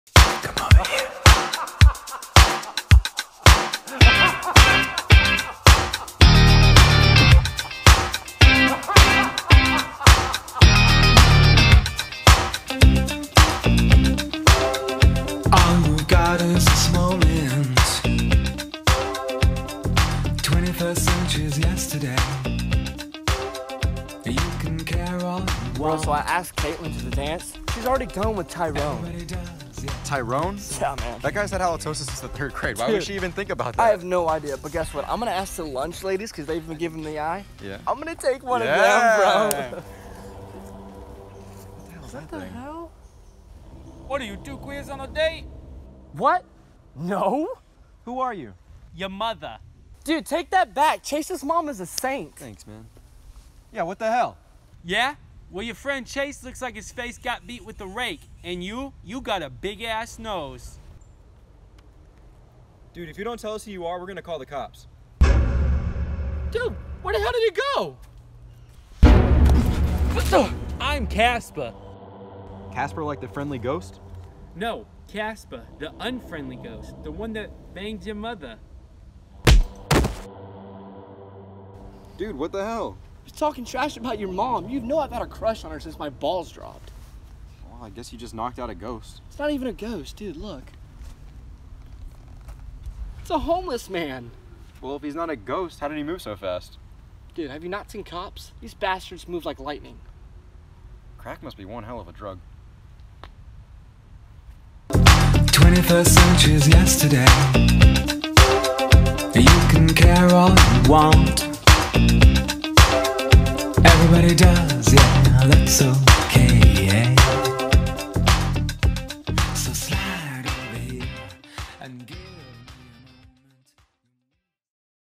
Casper, The Unfriendly Ghost | A Short Parody Horror Film (2018)
Chase & Ryan stumble across something ridiculous on a stroll through the woods. How cliche right?